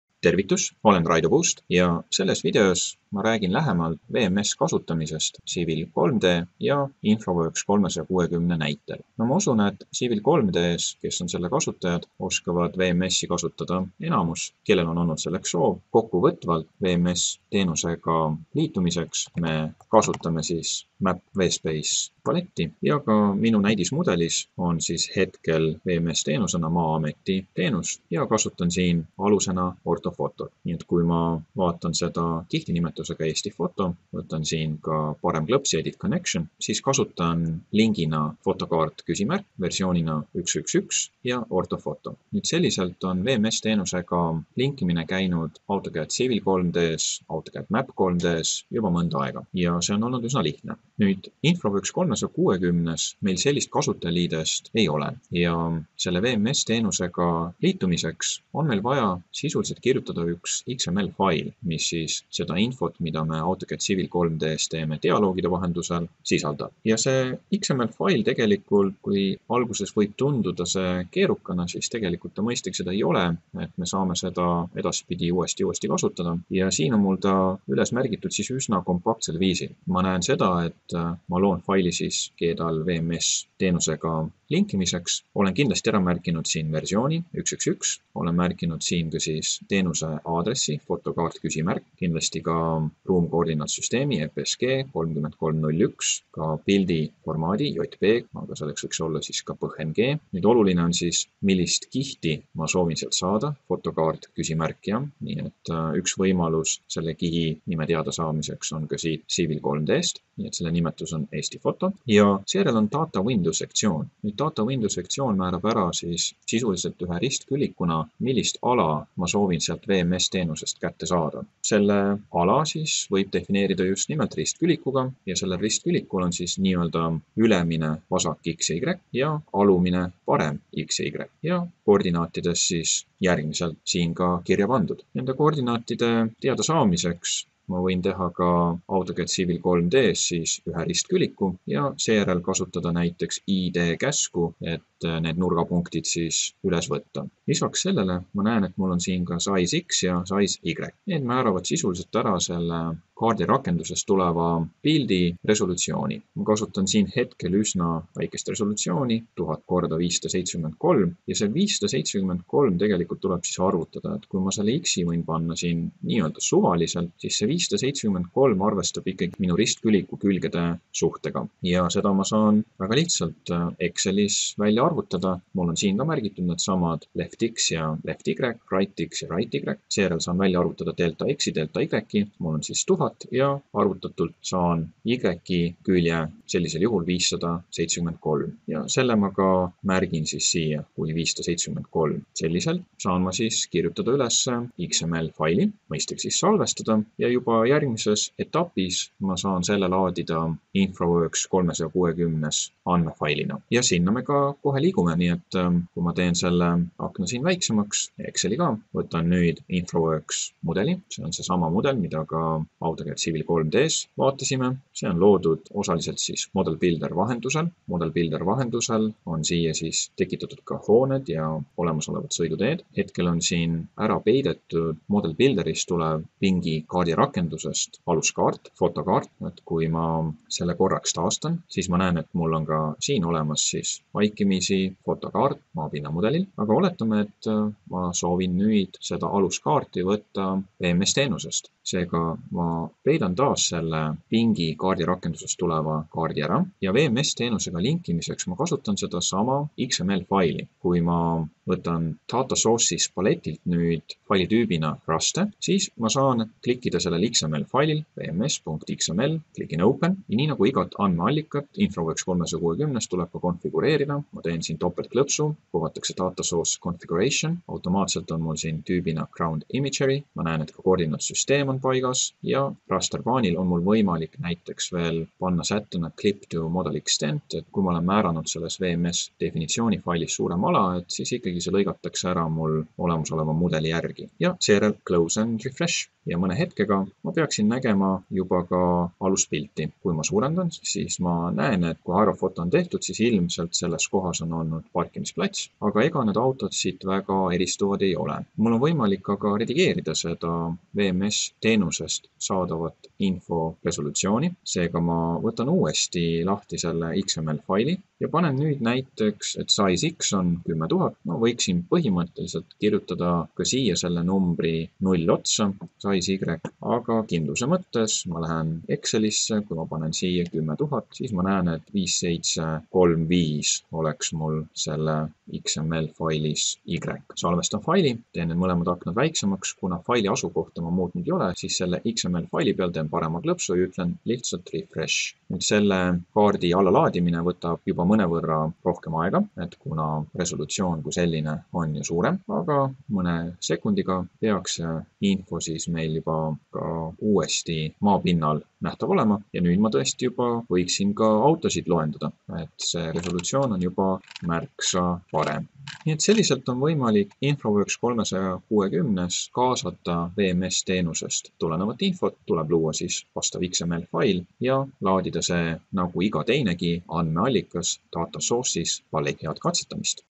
Autodesk InfraWorks 360 - Maa-ameti WMS teenuse kasutamine
Autodesk InfraWorks 360 võimaldab kaasata WMS teenusest tulevat kaardiandmestikku. Kui AutoCAD Map 3D / Civil 3D tarkvaras käib linkimine dialoogide vahendusel, siis InfraWorks 360 eeldab definitsioonifaili (*.xml) loomist.